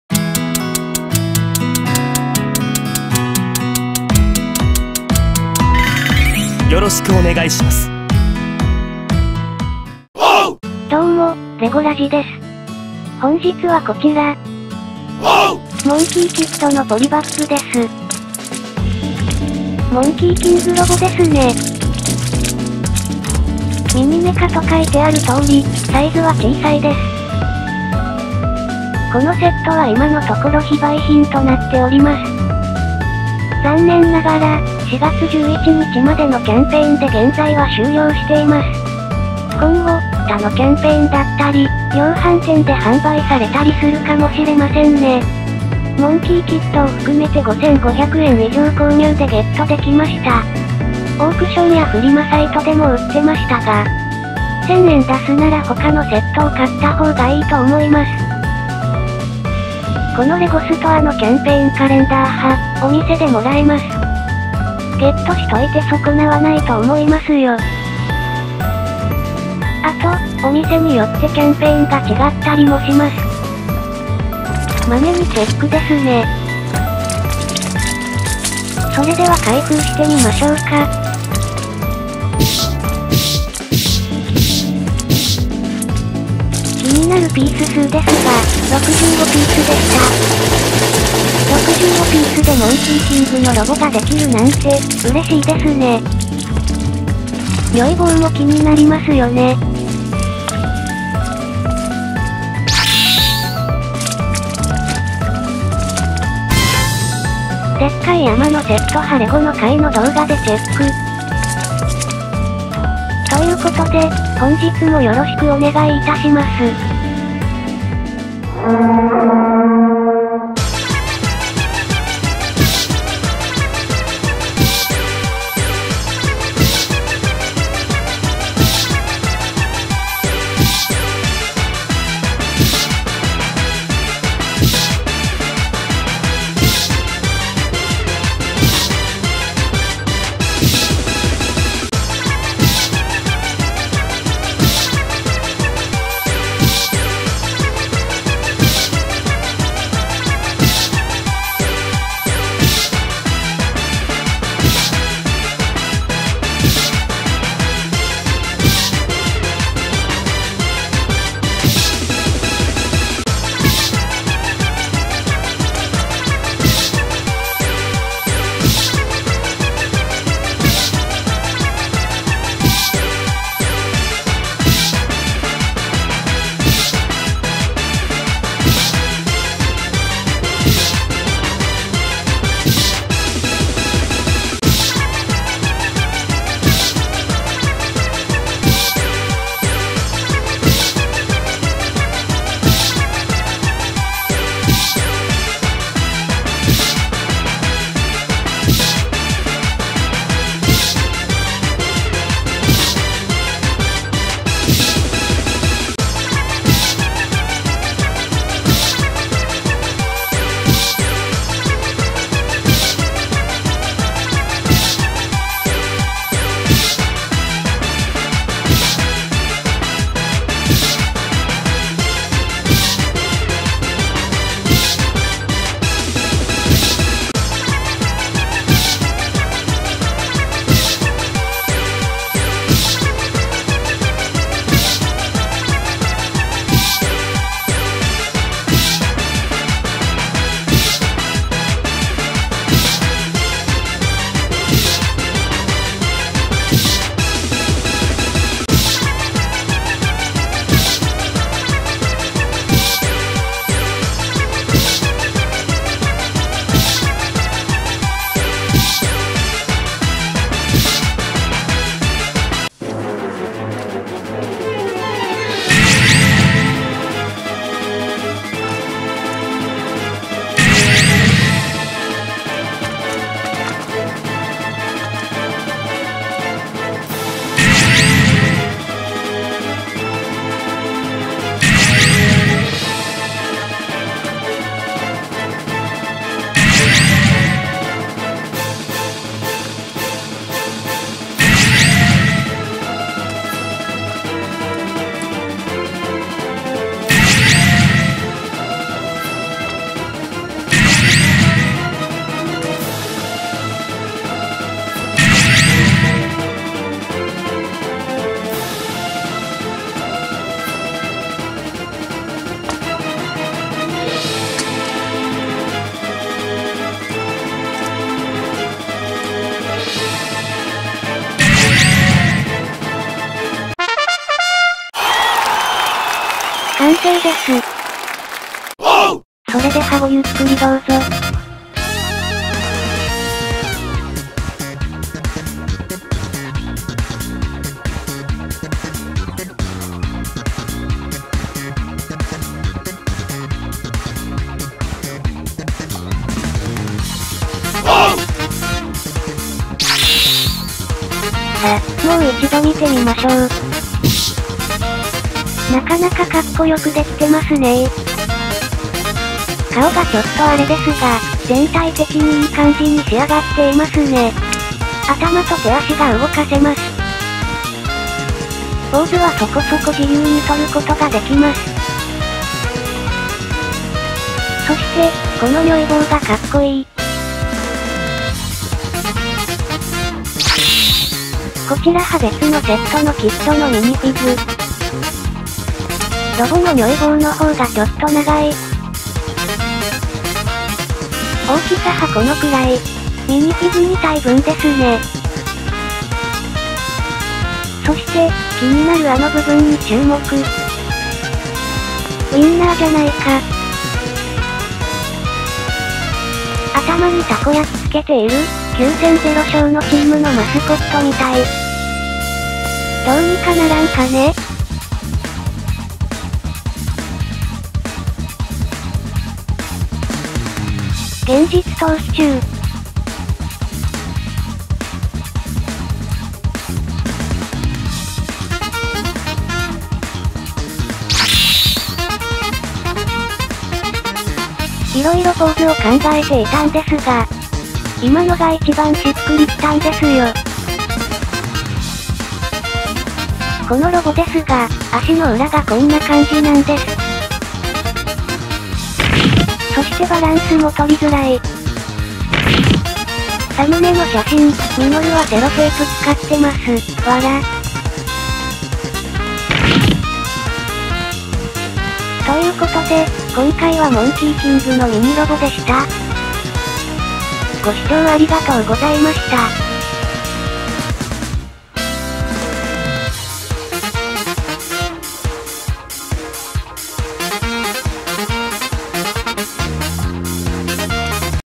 レゴ モンキーキッド モンキーキングメカ 30344 LEGO Mini Monkey King Warrior Mech
レゴストアのキャンペーンレゴです。
ミニフィグは付いておりませんが、ロボが組み立てられるポリバッグ。現在は終了したキャンペーンですが、またどこかでやるんじゃないかな？と思っています。いつもそうだし